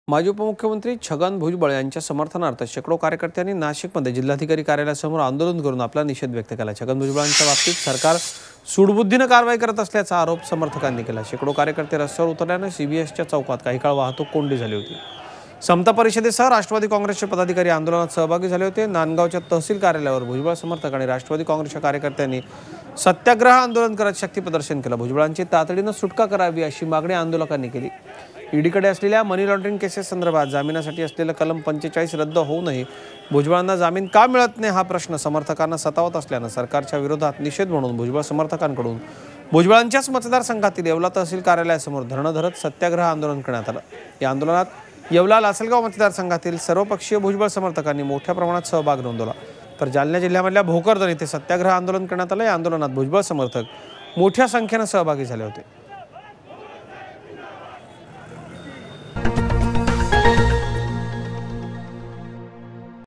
स्टेट | एनसीपीच्या भुजबळांसाठी कार्यकर्त्यांनी केलं आंदोलन
State | NCP Supporters Of Leader Chhagan Bhujbal Agitation For Release From Custody